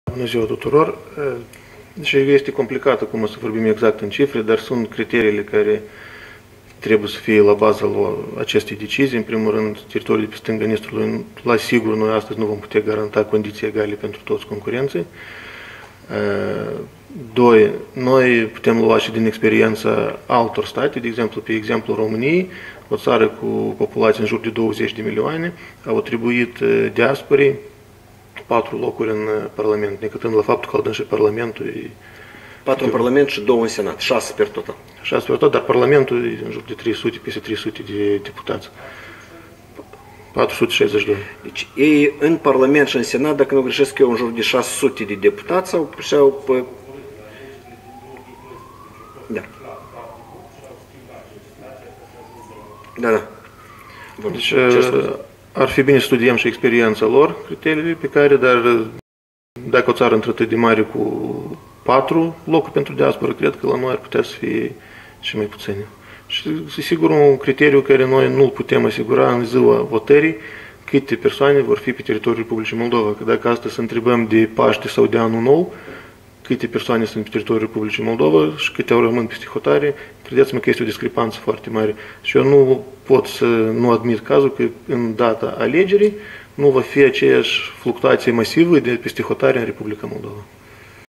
Ședința Comisiei Naționale pentru constituirea circumscripțiilor uninominale per...
Deci domnul deputat Nichiforciuc, cu &quot;vasta&quot; sa experienta prin diaspora propune sa acorde acesteia cit mai putine mandate in parlament pe baza experientii din Romania, care de fapt intr-un final a refuzat la sistemul electoral pe circumscriptii.
Titlul evenimentului: Ședința Comisiei Naționale pentru constituirea circumscripțiilor uninominale permanente